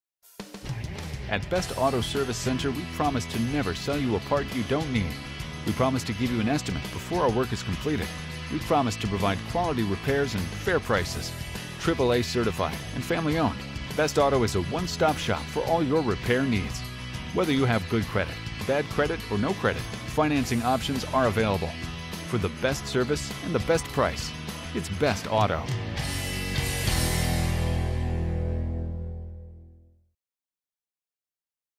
Commercial Production: Best Auto Service Center
Video shot for Marketing Agency.

Video was shot and directed by Park Multimedia, the agency wrote the script and edited. Not master copy pulled from an online source.